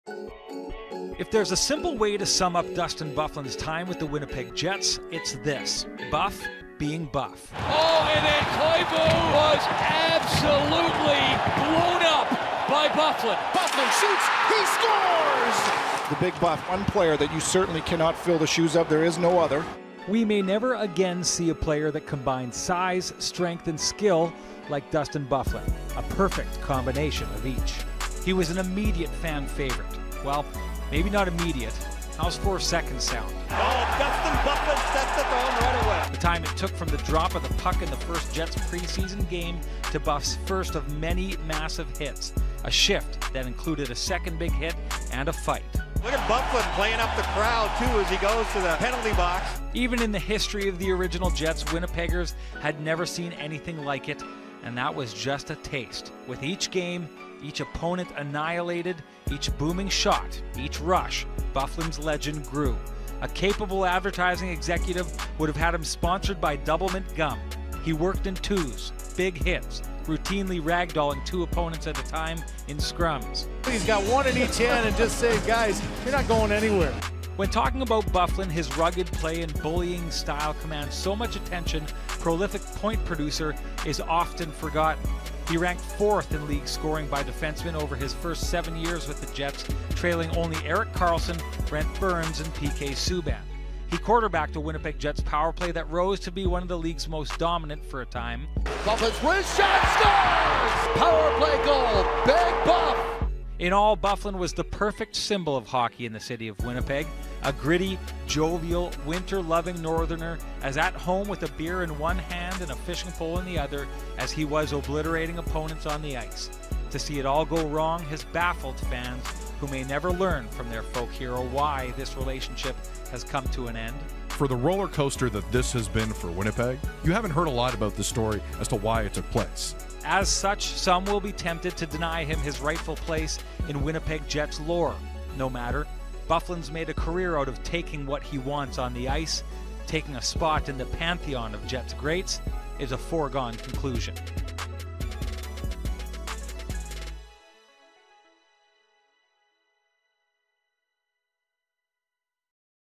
Looking Back At Dustin Byfuglien’s Best Moments With The Winnipeg Jets
With his time in Winnipeg officially coming to an end, Sean Reynolds looks back at some of the best moments in Dustin Byfuglien's amazing career with the Jets.